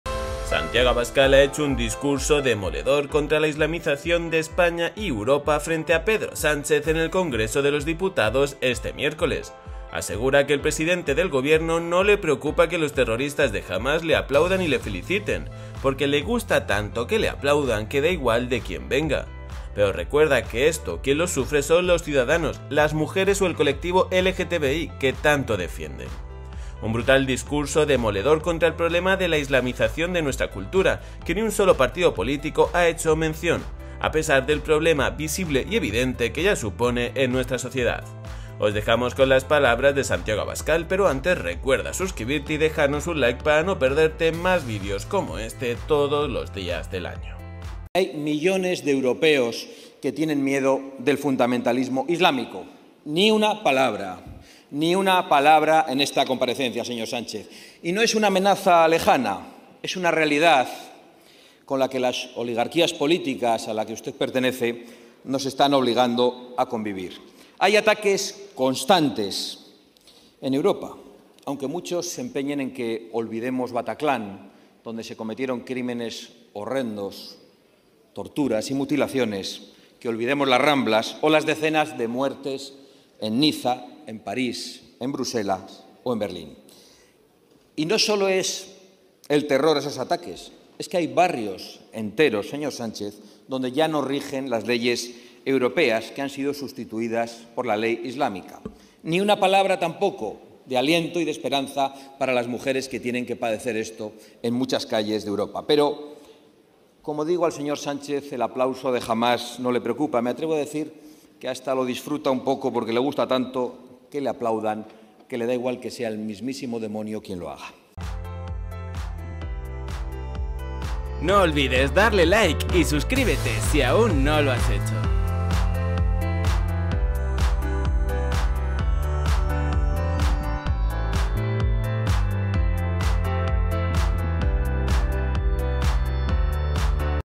Abascal DENUNCIA LA ISLAMIZACIÓN de Sánchez: “TE GUSTA HAMAS”
Santiago Abascal ha hecho un discurso demoledor contra la islamización de España y Europa frente a Pedro Sánchez en el Congreso de los Diputados.

Asegura que al presidente del Gobierno no le preocupa que los terroristas de Hamas le aplaudan, porque le gusta tanto que le aplaudan que da igual de quien venga.
Pero recuerda que esto quien lo sufre son los ciudadanos, las mujeres o el colectivo lgtbi que tanto defienden.

Un brutal discurso contra el problema de la islamización de nuestra cultura que ni un solo partido político ha hecho mención, a pesar del problema visible que ya supone en nuestra sociedad.